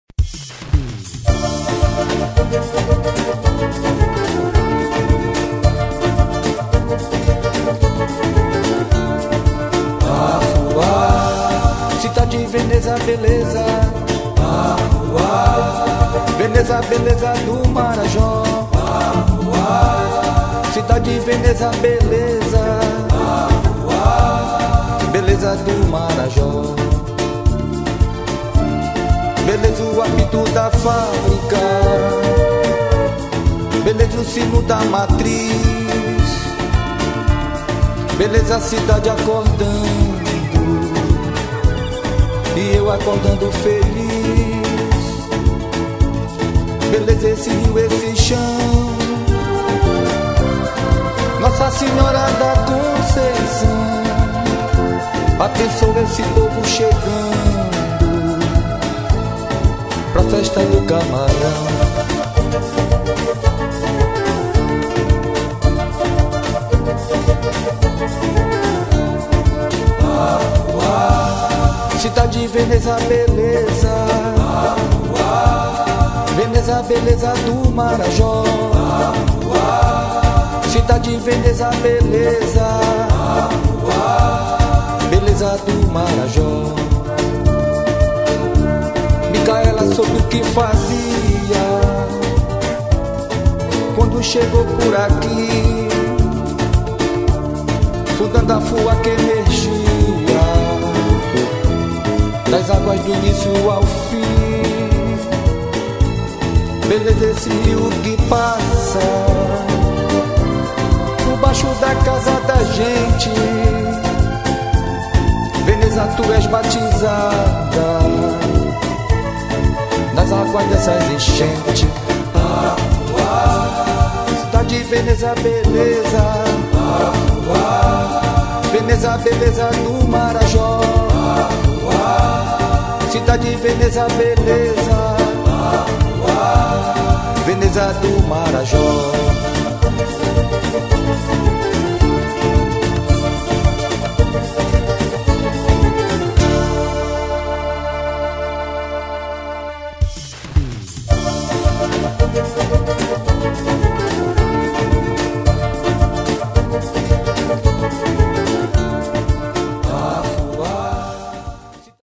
E a cidade das pontes, com mais uma rua.
fotoselugares
Ilha de Marajó
Afua,. A Veneza  Marajoara.  Cidade das pontes, bicicletas e com sua arquitetura única.
Minha cidade, minha rua.
Bairro Cidade Nova
Visite Afuá -  Compartilhe, deixe seu comentário e de seu like. Visite o Canal YouTube Edivan Afua.